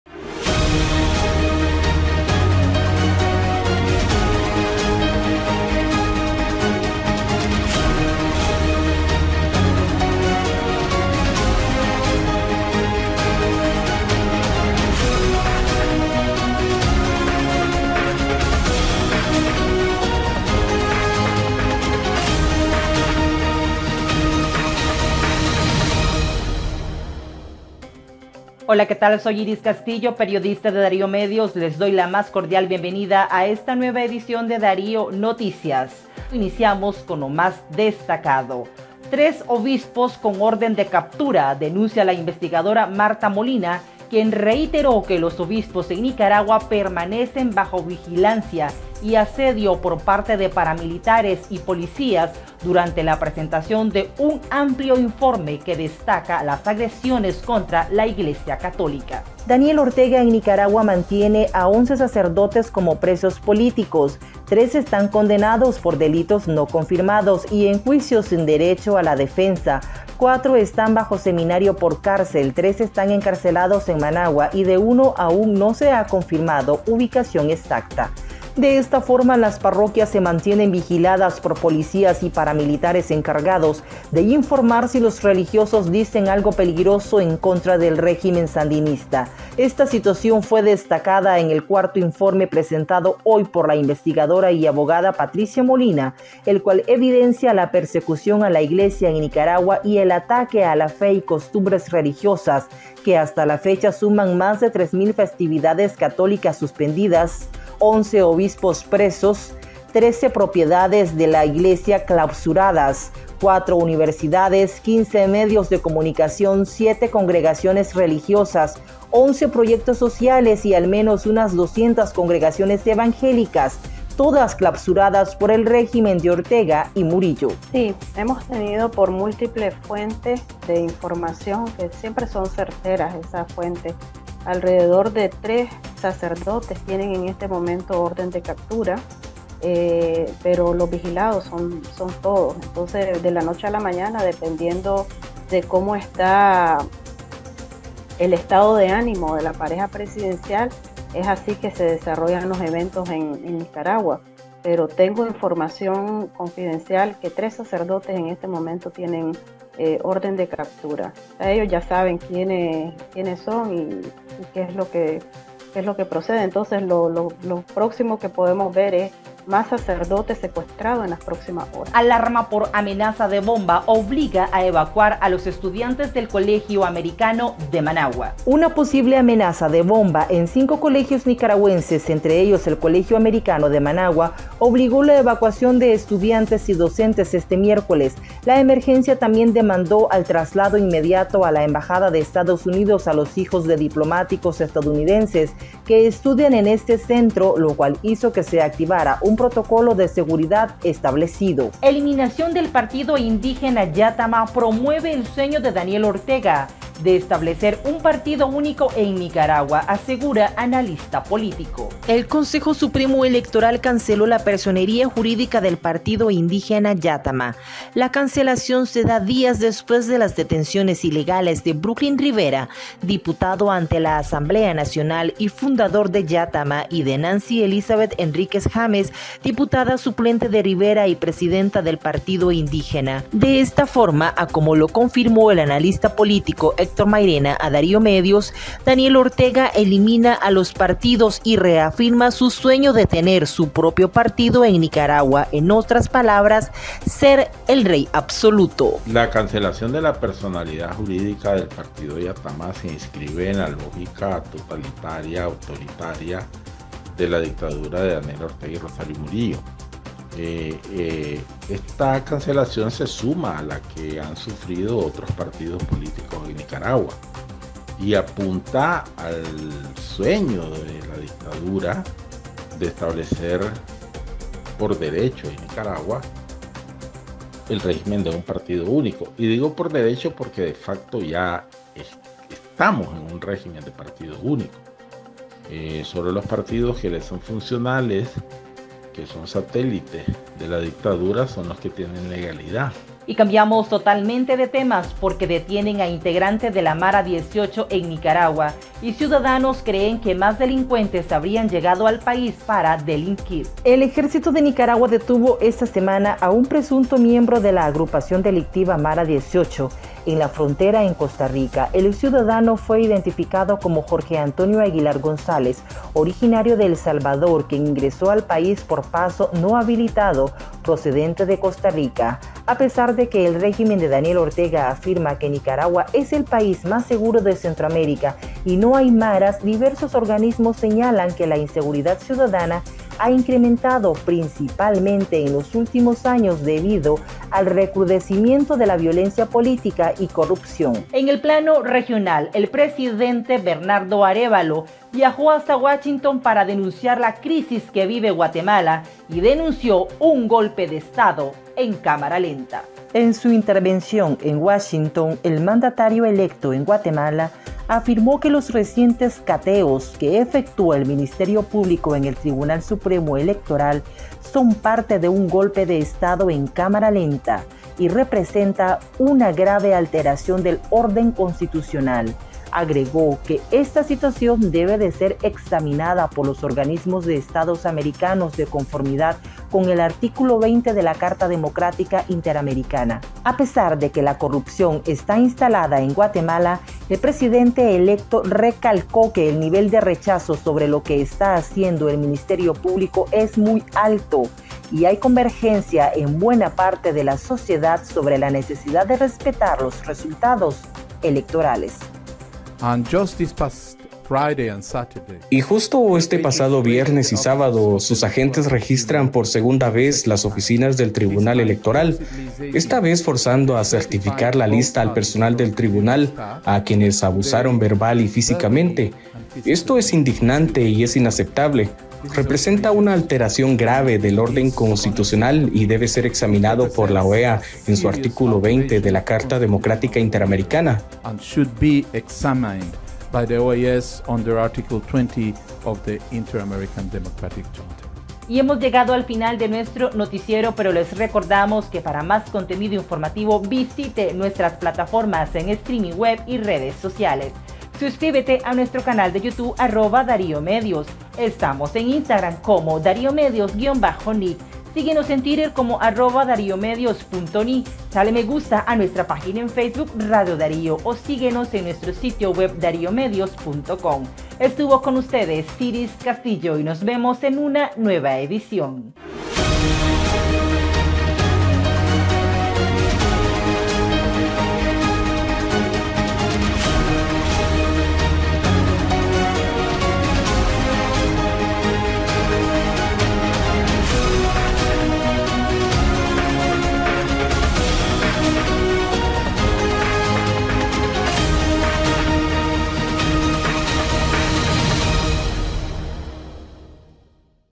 Orden de captura contra otros tres obispos, denunció la investigadora Martha Patricia Molina
Orden de captura contra otros tres obispos, denunció la investigadora Martha Patricia Molina tras reiterar que los sacerdotes en Nicaragua permanecen vigilados por paramilitares y policías. Esto y más en el noticiero de DarioMediosInternacional